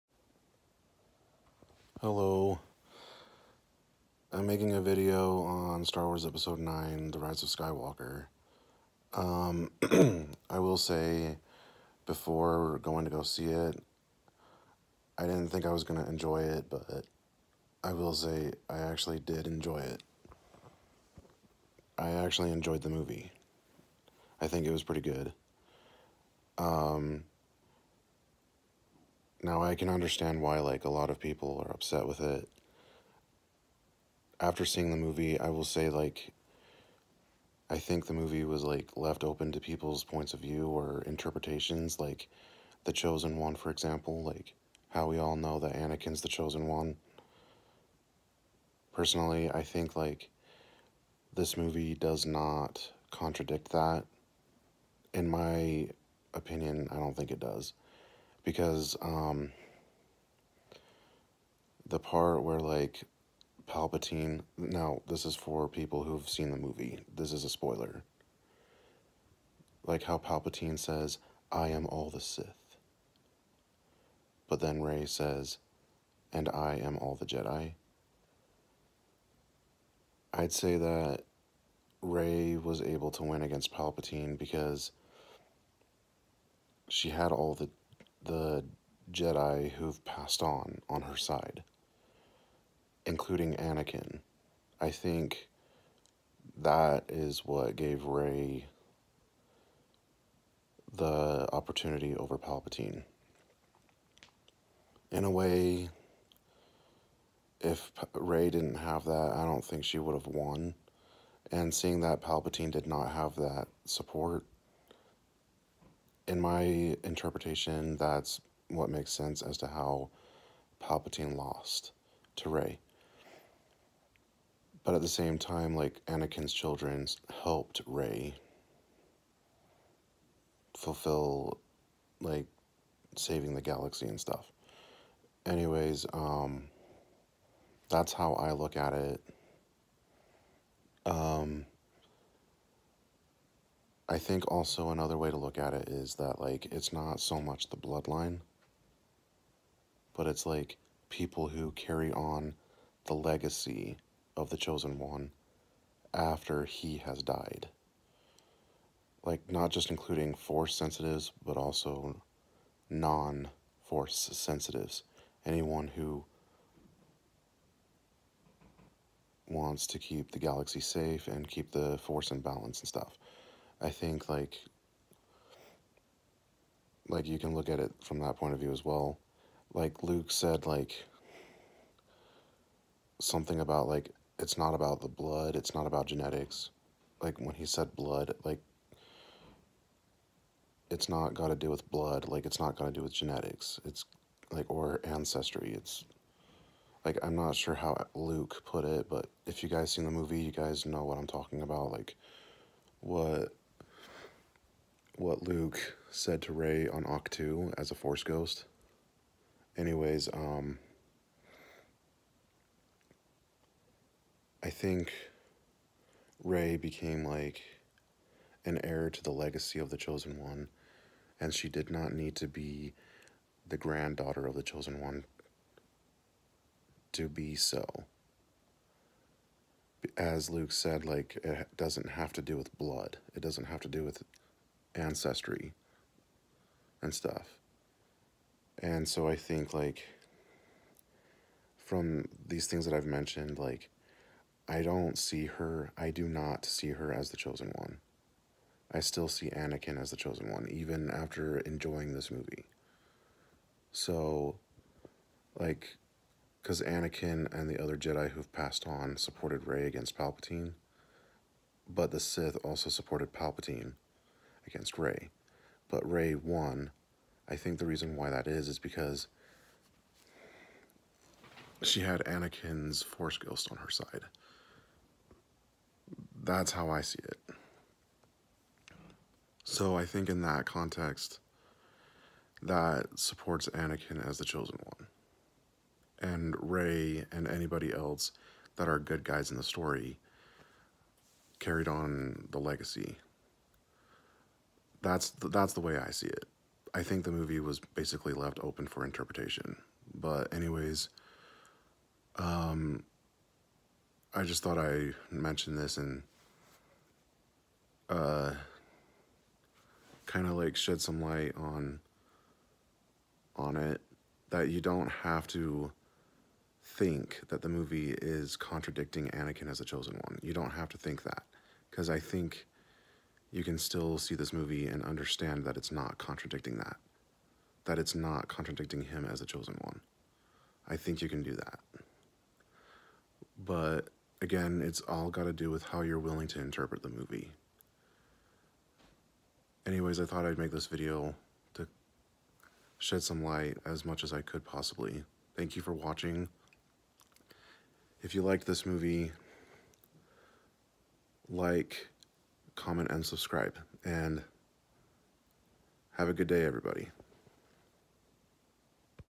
For Those Who’ve Seen Star Wars The Rise of Skywalker (Spoiler warning)
Sorry if I’m really quiet in this movie.  It was like 1:30 in the morning, and I wanted to be quiet so I didn’t wake anyone.  I think Star Wars: Episode IX-The Rise of Skywalker was left open to the personal interpretations of the fans, and I personally interpret it as Anakin aiding the living from the t
world of force ghosts.  Palpatine said “I am all of the Sith” and Rey said “And I am all of the Jedi”; how I see it is that Rey had the upper hand against Palpatine because of the Jedi who were already passed on.  Anakin Skywalker was among those Jedi, and all of the dead Jedi aided her while supposedly all of the dead Sith aided Palpatine (I think that might have been).  What makes the two different though is Rey had Anakin’s support, Palpatine did not.  Rey was able to obliterate Palpatine because of that, at least in my own personal interpretation.  In this point of view, I see the movie supporting Anakin as the chosen one.  I think though this movie left it open for the fans to interpret it however way they will.  However, this is how I see it, with Rey having help from the dead Jedi, especially Anakin.  I wish Anakin made an appearance in the movie, but I’m overall happy with what we got.